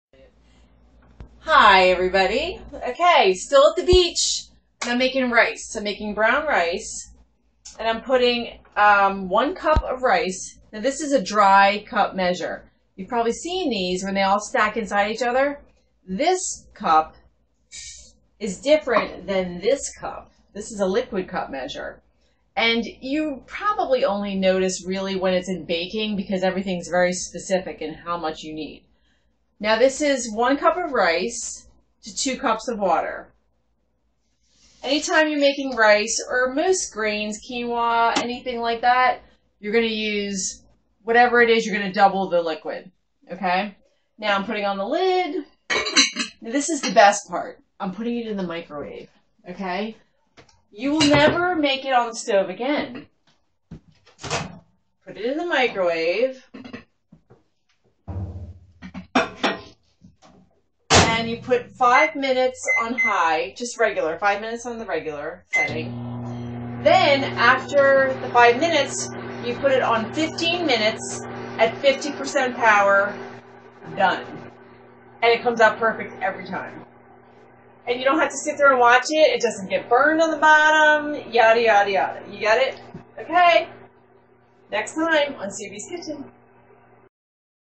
Perfect Rice every time!
Microwave Rice or Quinoa – EASY

1 cup rice or quinoa
2 cups water

Place rice or quinoa (rinse quinoa first) in a microwave safe dish (at least a 2 quart dish).  Microwave on high for 5 minutes, then microwave for 15 minutes at 50% power.  Fluff with a fork and serve – if using brown rice, set time for 25 minutes on 50% power.  If a little water still remains, set another 5-10 minutes on 50% power.